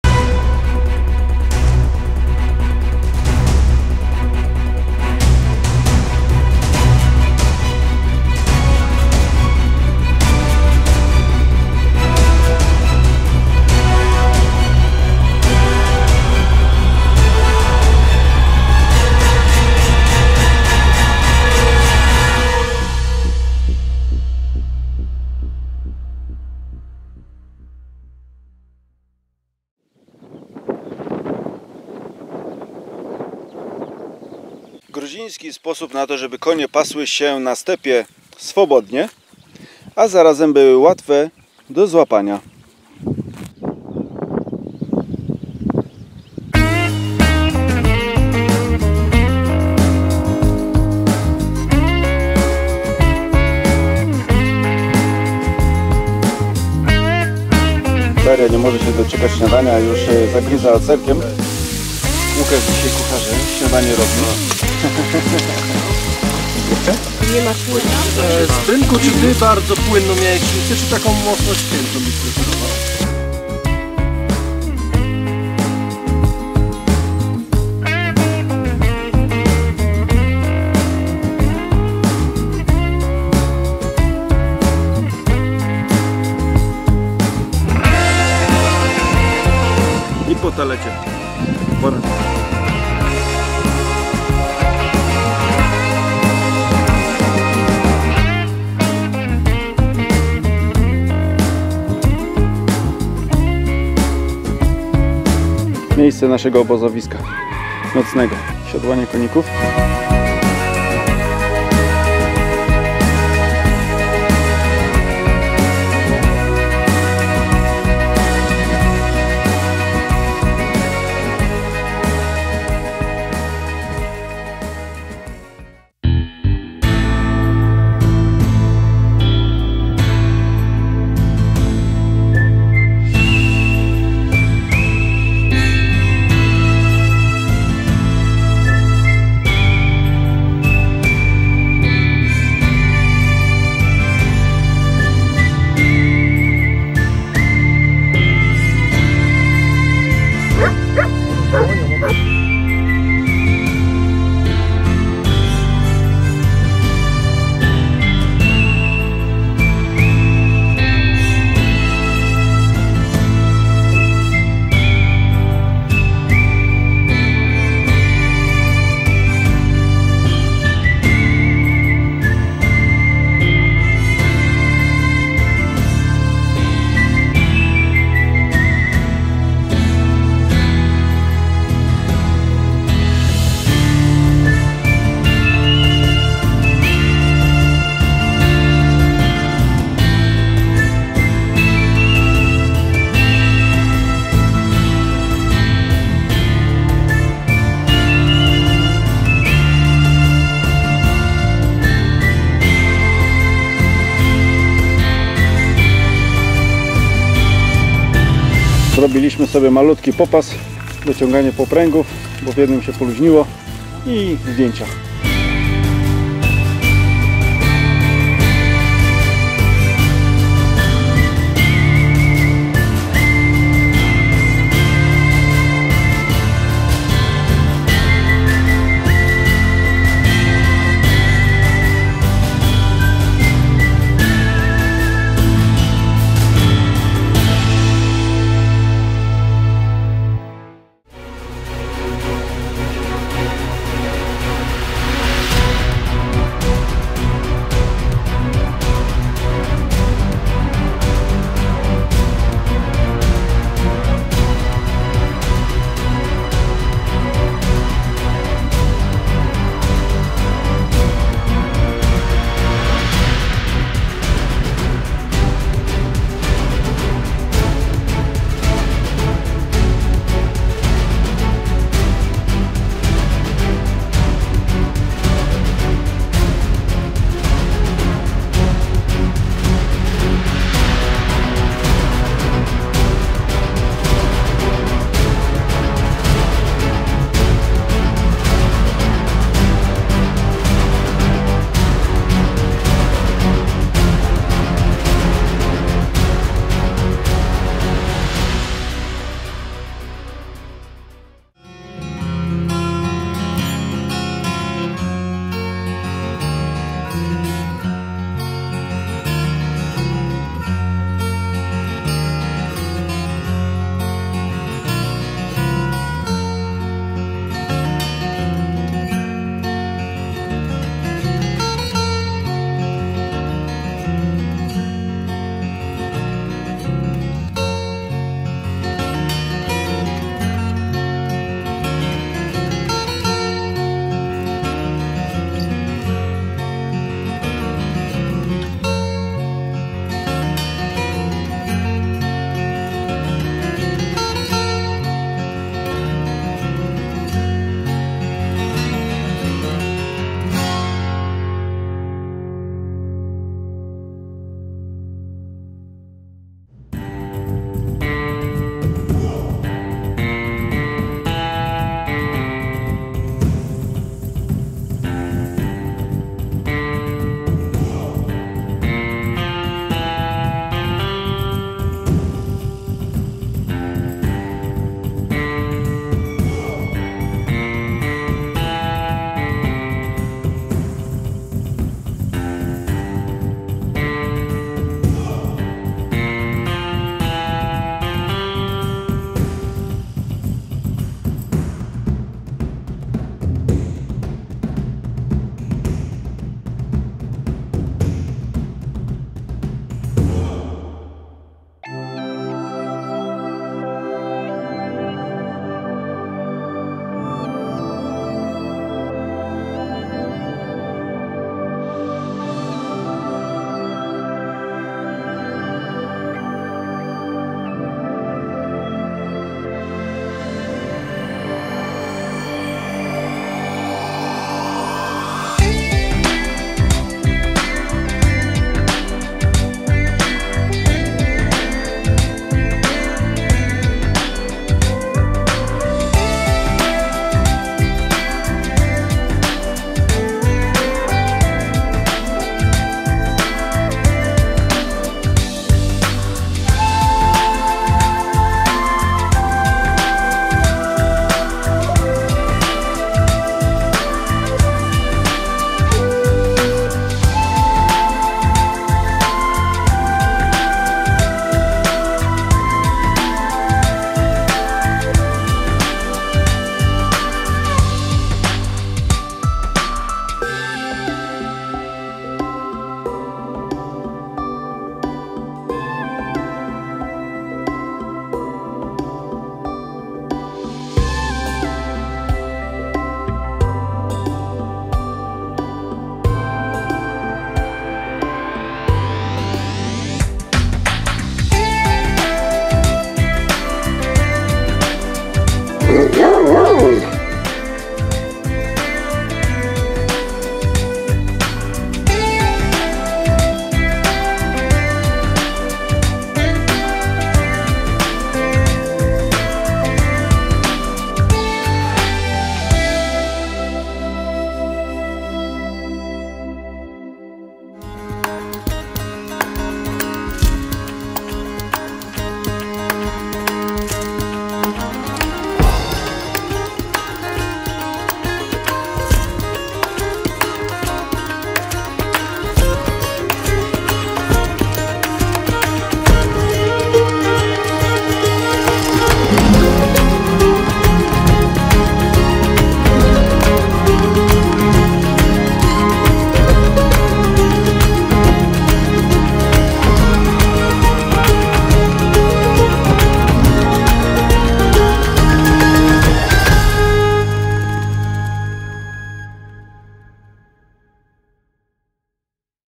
Czarna Góra, Gruzja, part 3
Pierwszy dzień konnej wyprawy. Jedziemy 25 kilometrów na Czarną Górę. Jesteśmy blisko granicy z Azerbejdżanem.
     Wydawało by się, że dzicz. Jednak samochody wjeżdżały na tą górę.
     Wysokość góry nigdzie nie była podana, lecz na tablicy informacyjnej przeczytałem, że ma szatę roślinną typową dla wysokości 500-700 m n.p.m. Koniki faktycznie musiały się napracować.

     The first day of a horse trip. We drive 25 kilometers to Czarna Góra. We are close to the border with Azerbaijan.
      It would seem that wilderness. However, the cars were driving up this mountain.
      The height of the mountain was nowhere specified, but on the information board I read that it has a typical vegetation of 500-700 m above sea level. Koniki actually had to work hard.